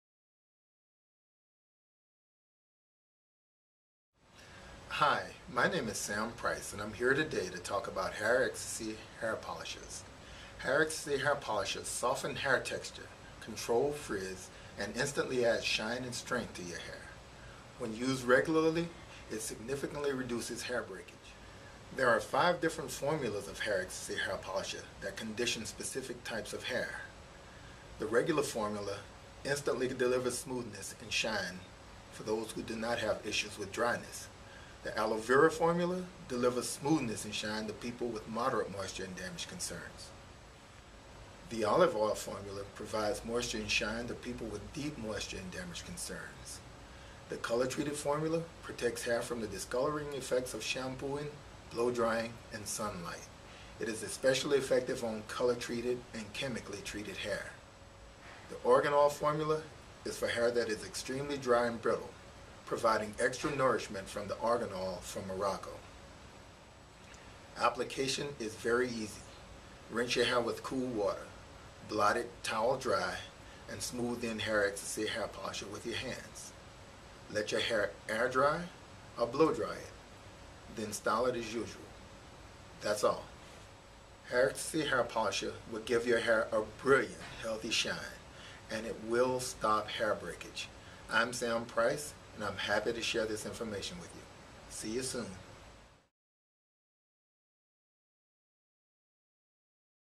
Hair Ecstasy - Hair Polishers - Aloe Vera - Olive Oil - Argan Oil - Sam Pryce - English - Ingles
In this video Sam tells us about the different kinds of Hair Polisher and for what type of hair each one is appropriate.
He also explains how to apply the polisher to the hair.
Comment if you have questions.